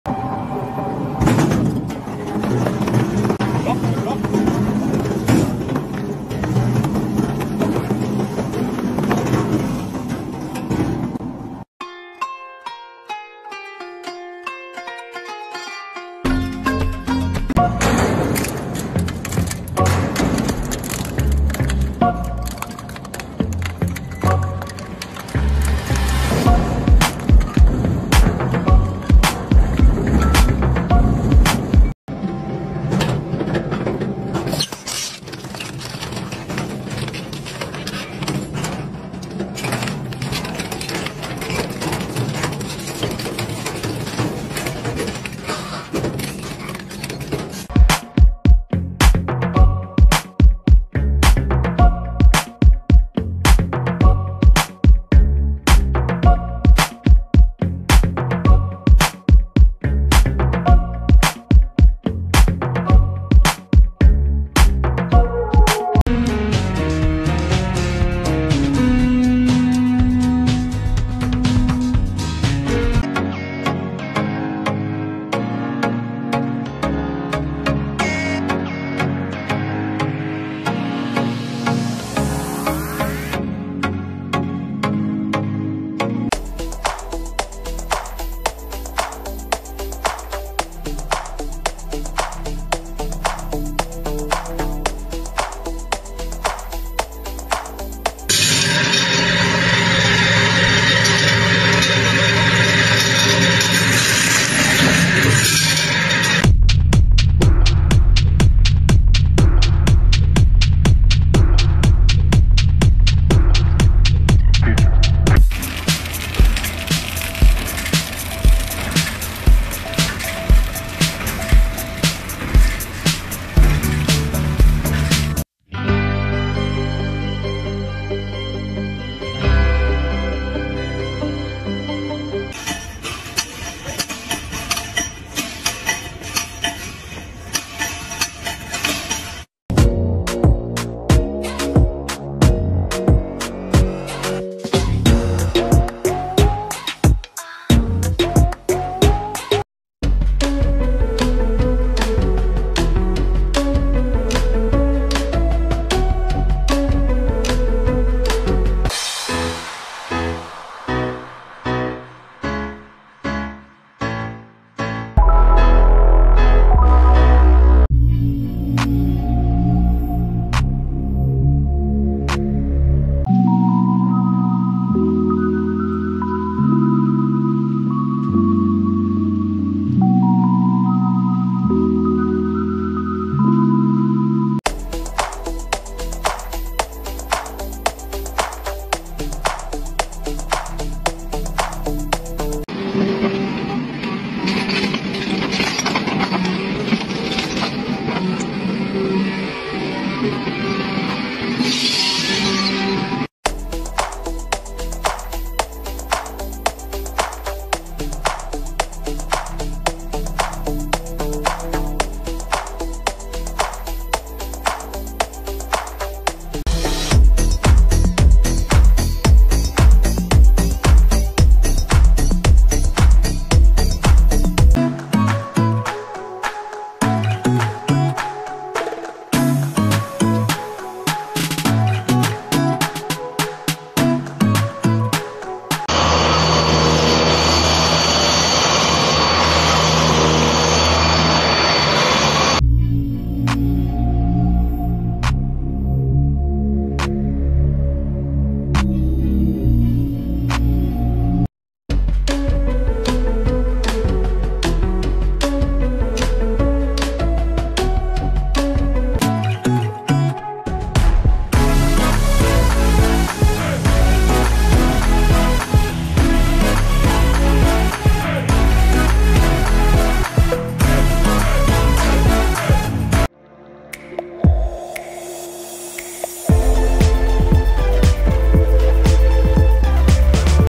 Most Enjoyable and Satisfying Machines and Factory Jobs
Welcome back to another video from GlobalTechnology!

😀 Today, we'll be going over a number of cool production processes and covering what they make!
Prepare to get your mind blown by next-level life-hacks, innovative machines, smart gadgets and super satisfying tips on how to get jobs done in a skillful way.
Not to mention amazing tools and ingenious inventions doing stuff that’s so visually appealing that will leave you in a state of shock. Workers operating at another level with woodworking projects and construction jobs.
✔We hope you enjoy this review of  Most Enjoyable and Satisfying Machines and Factory Jobs